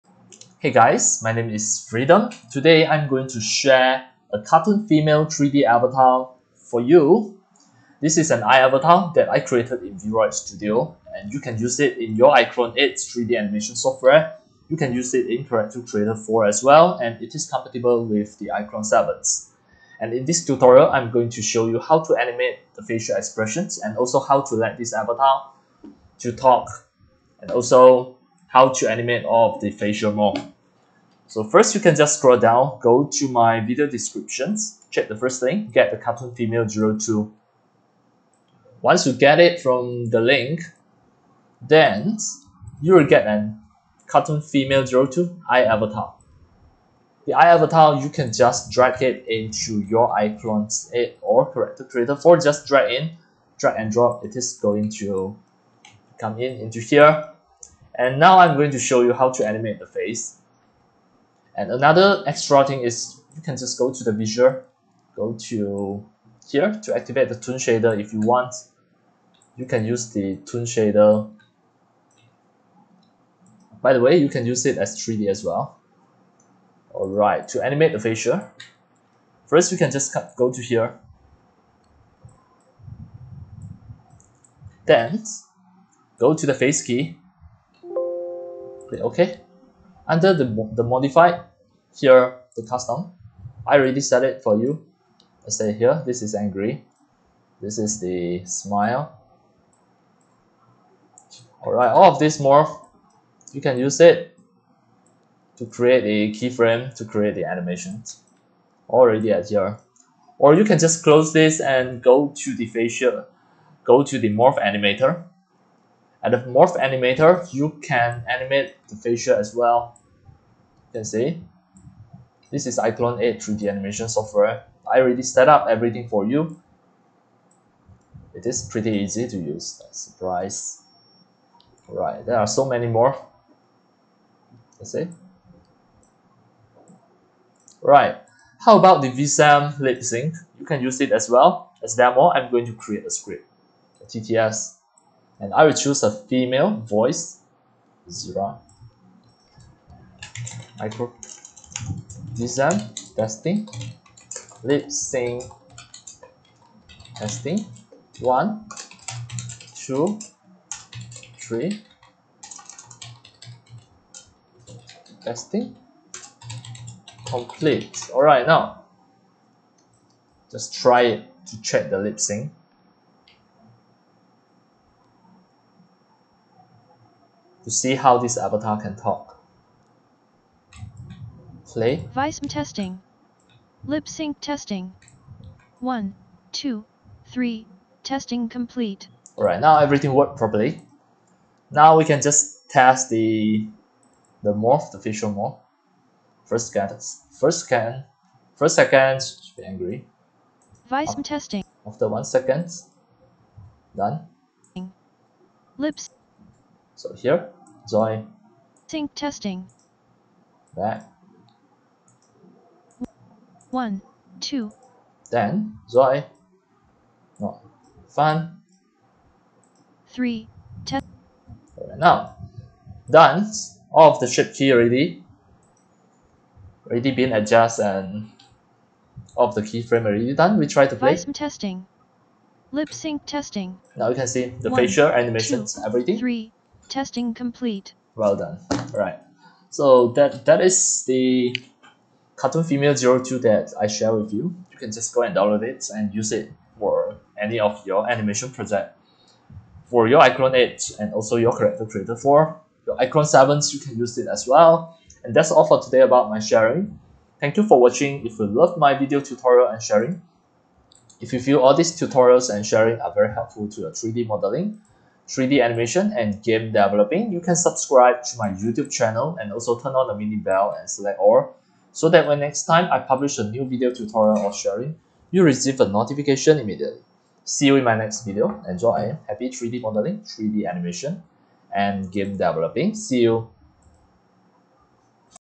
Freedom 3D Cartoon Female 02 to iClone 8 - iAvatar Sharing
- Hey guys, this is a manga cartoon 3D avatar female I created in Vroid and now I already converted her as iAvatar with lip sync, facial and viseme for your iClone 8 3D Animation! In this tutorial, I am going to show you how to animate her in iClone. ENJOY!

Get iClone 8 and CC4 Contents

Your friend,
FREEDOM